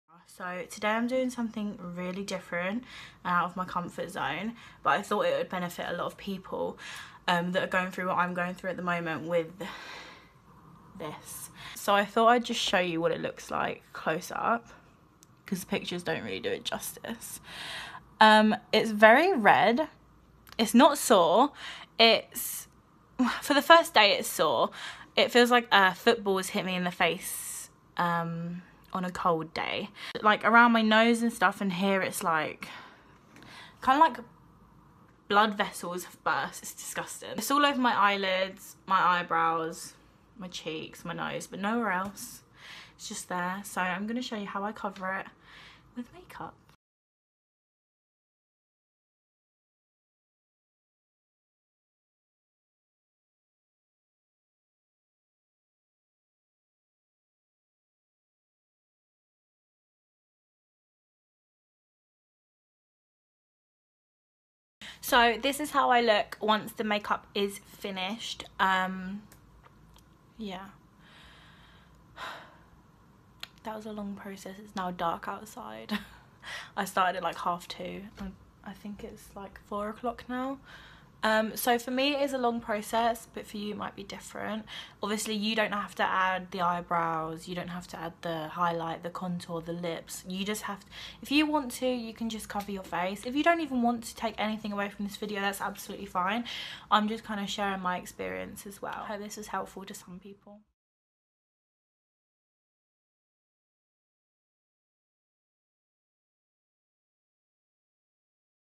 Cruelly bullied teen reveals her 90 minute makeup routine to hide rash
Chloe Boweren, from Stanwell, Surrey, was cruelly bullied when the malar rash first appeared across her eyes, nose and cheeks three years ago (left). The teenager spends up to 90-minutes covering it with make-up (centre), and has shared her routine in the hope of helping other people with lupus. Right: Chloe wearing make-up to cover the rash.